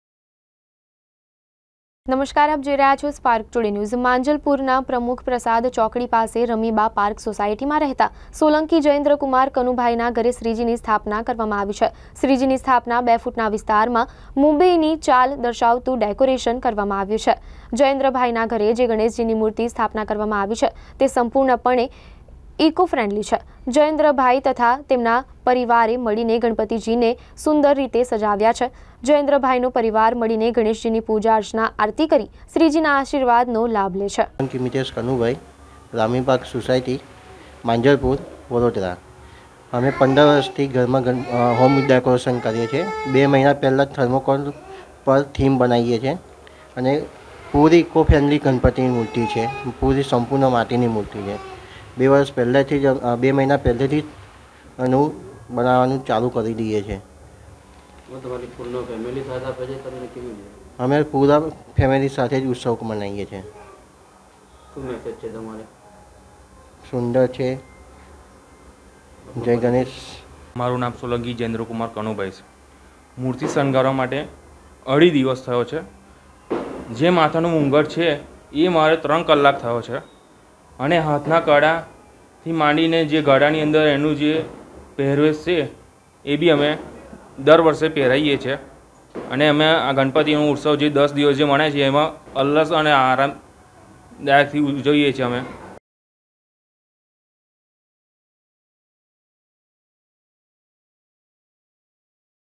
સોલંકી જયેન્દ્ર કુમાર કનુભાઈ ના ઘરે શ્રીજીની ઇકો ફ્રેન્ડલી પ્રતિમા ની સ્થાપના કરવામાં આવી 2024
સોલંકી જયેન્દ્ર કુમાર કનુભાઈ ના ઘરે શ્રીજીની ઇકો ફ્રેન્ડલી પ્રતિમા ની સ્થાપના કરવામાં આવી